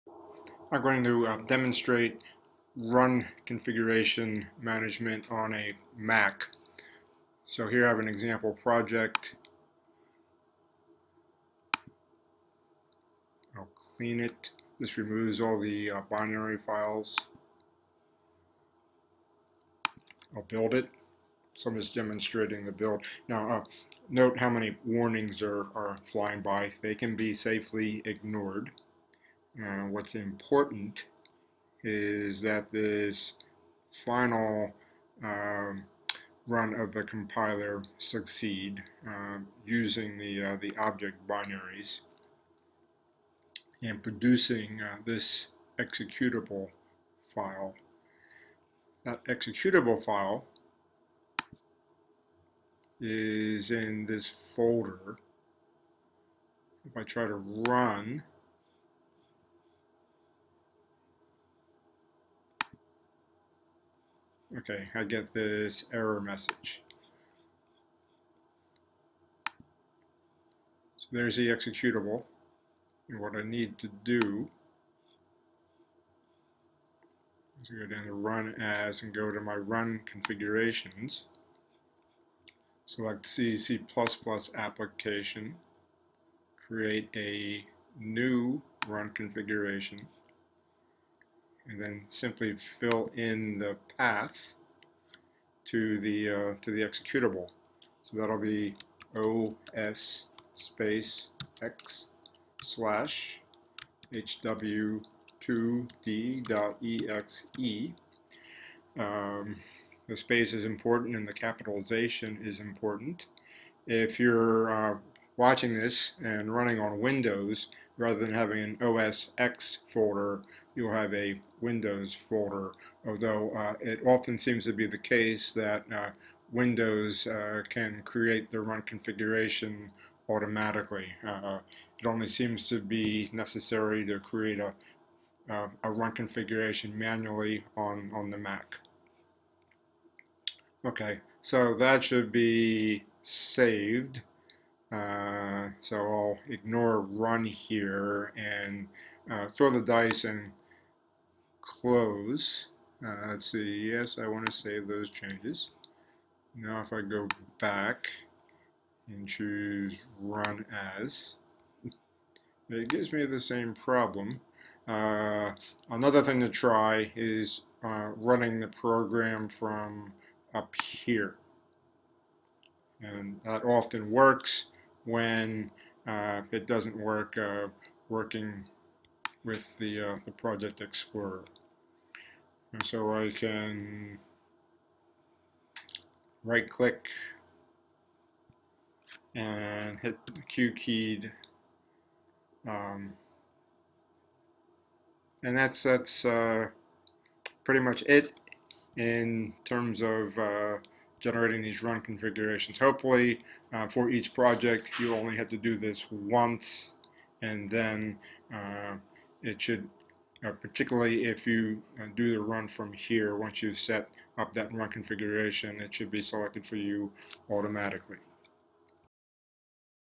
Managing Eclipse Run Configurations on a Mac
This how-to demonstrates creating an Eclipse run configuration on a Mac, which doesn't appear to do so automatically, as is the case on Windows.